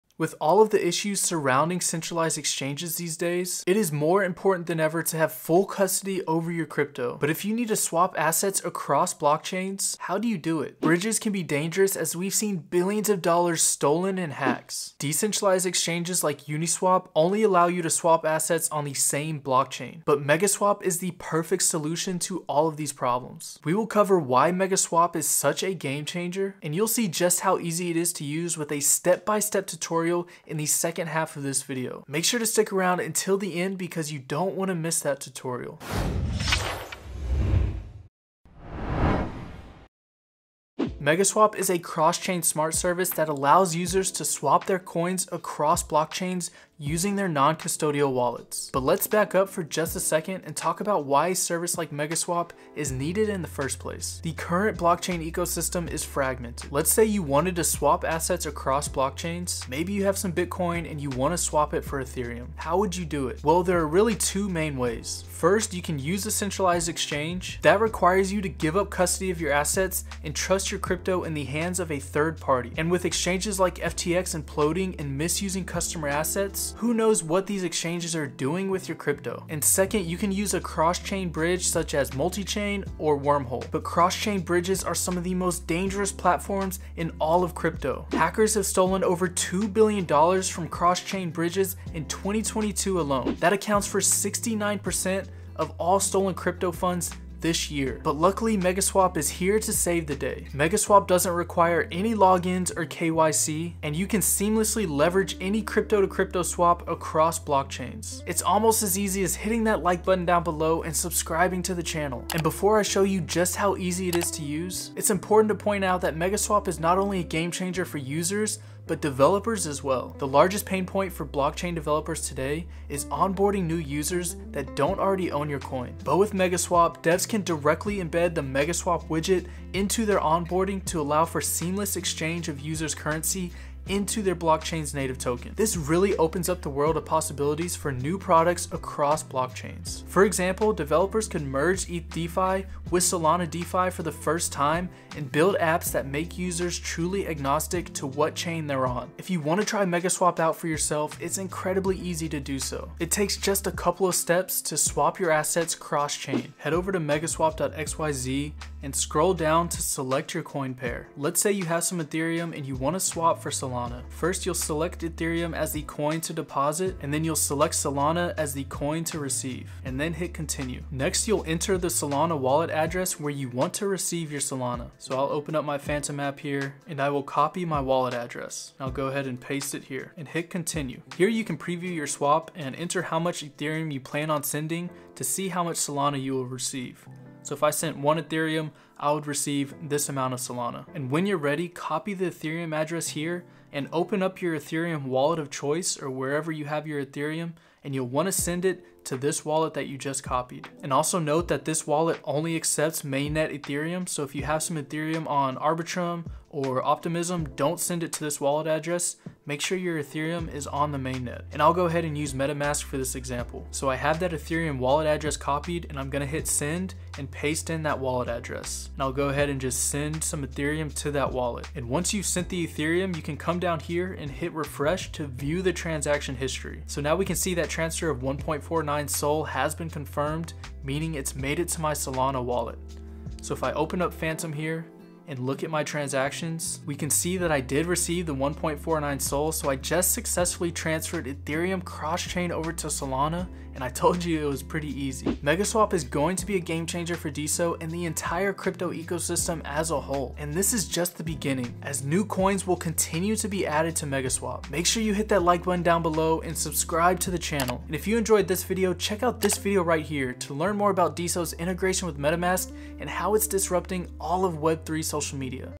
How to Swap Crypto Across Blockchains With Ease | MegaSwap Tutorial
With all of the issues surrounding CEXs these days, it is more important than ever to keep full custody of your coins.

But if you need to swap assets across blockchains, how do you do it? Bridges can be dangerous as we have seen BILLIONS of dollars stolen in hacks.

DEXs like Uniswap only allow you to swap assets on the same blockchain.

But Megaswap is the perfect solution to all of these problems.

MegaSwap is a Cross-chain smart service that allows users to swap coins between blockchains using their non-custodial wallets.

Time Stamps:
0:00 Intro
0:40 The Problem with Cross-Chain Swaps
2:29 MegaSwap Tutorial
4:05 One Last Thing…

Learn more about DeSo:
Diamond